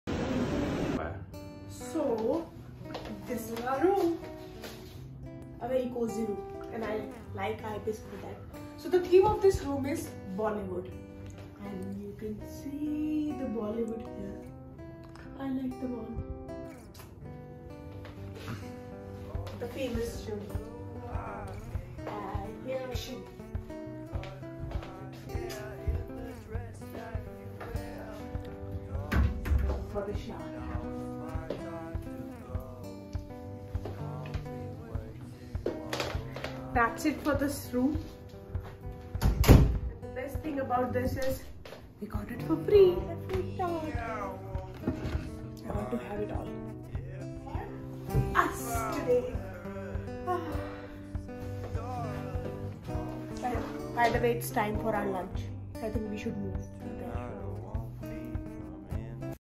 HOW WE GOT A FREE STAY AT IBIS AEROCITY?
We were elated to know that we won a free stay at Ibis Aerocity Hotel recently including all meals and beverages. The hotel is located next to both T1 and T3 airport terminals.  Centrally located, the hotel is connected by NH8 linking it to Cyber City, Cyber Hub and Ambience Mall. The hotel is next to Worldmark where you have ample amount of options to even dine out!

Staying at ibis New Delhi Aerocity Hotel means easy access to New Delhi, a dynamic, exciting city that is constantly evolving!

The hotel is pet friendly! and have the Longest breakfast from 4am to Noon.